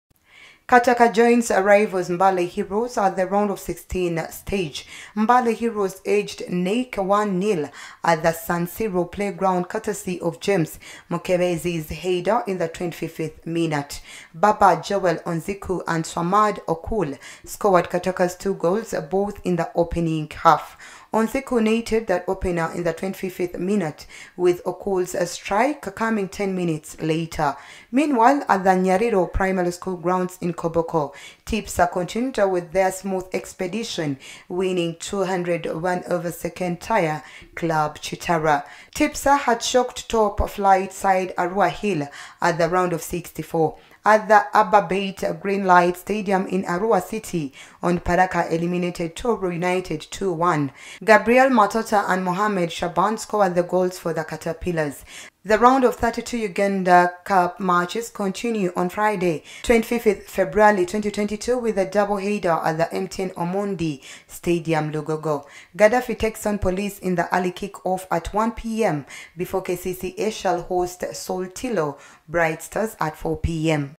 Kataka out-smart Paidha Black Angels to qualify for round of 16
FUFA Big League outfit Kataka bundled out visiting West Nile regional side Paidha Black Angels 2-0 in the Stanbic Uganda Cup at Mbale Municipal Stadium on Wednesday, February 23, 2022.
Kataka joins rivals Mbale Heroes at the round of 16 stage.  Mbale Heroes edged NEC 1-0 at the SanSiro playground courtesy of James Mukebeezi’s header in the 25th minute.